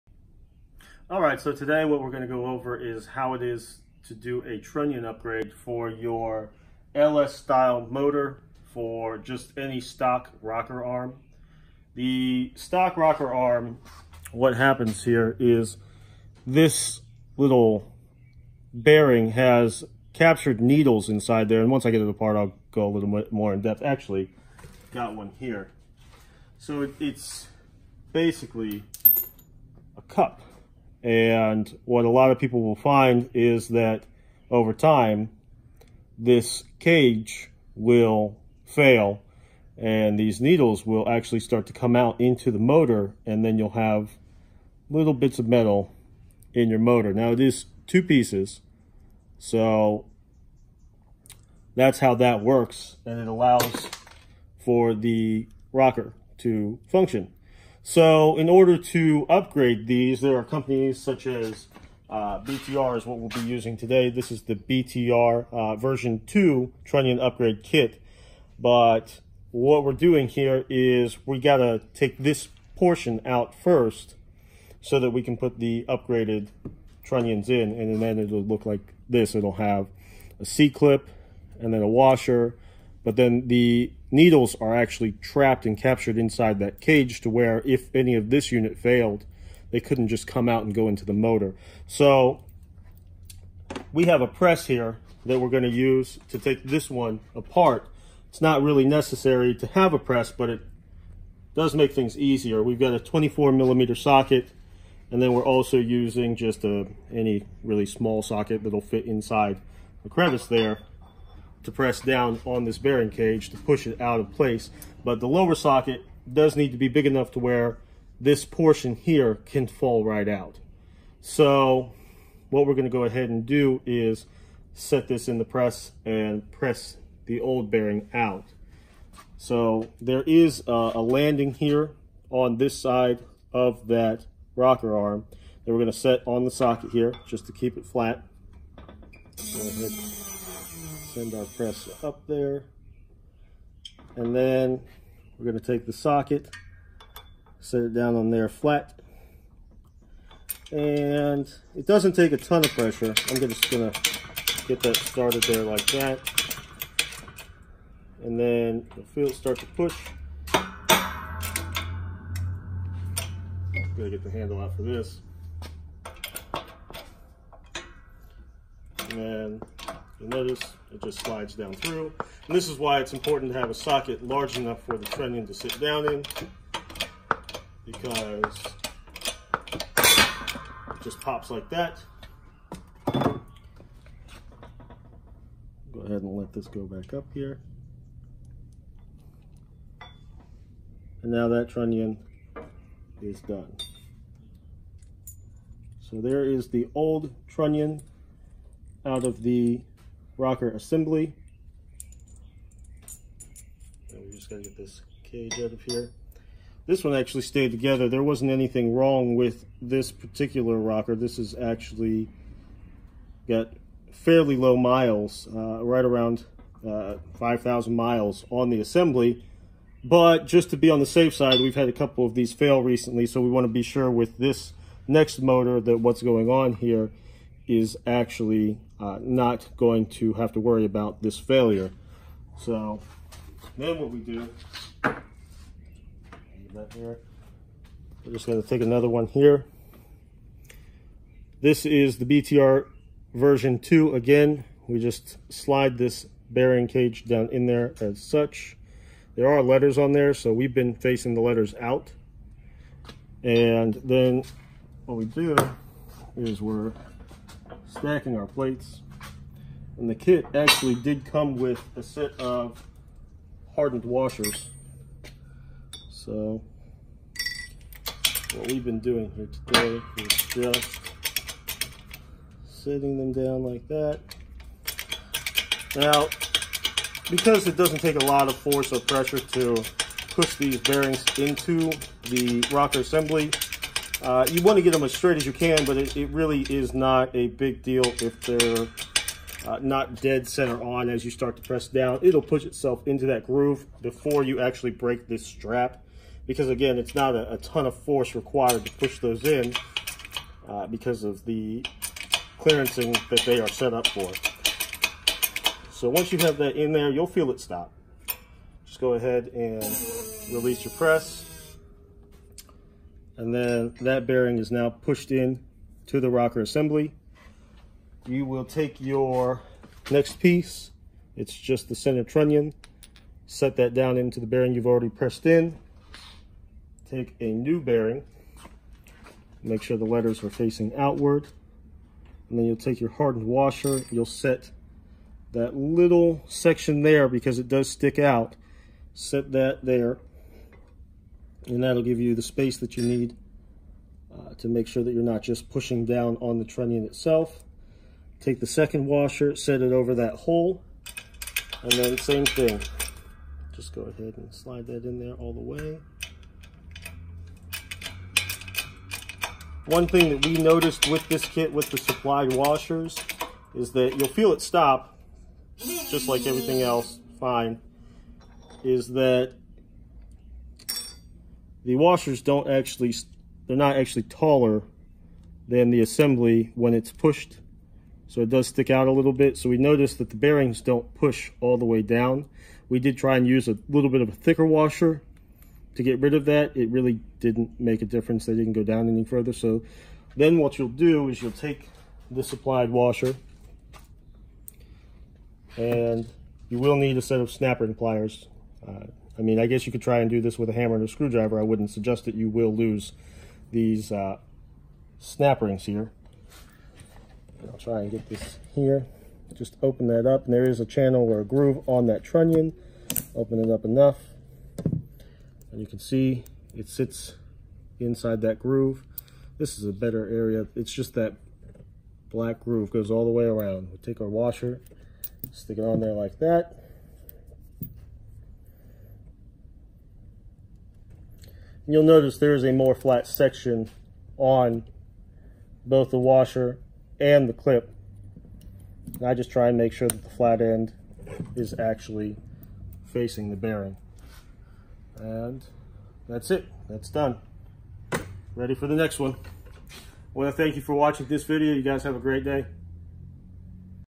How to BTR LS rocker trunnion assembly
In this video we demonstrate a method to disassemble the factory rocker and trunnion and upgrade to the BTR Trunnion on an LS style rocker arm.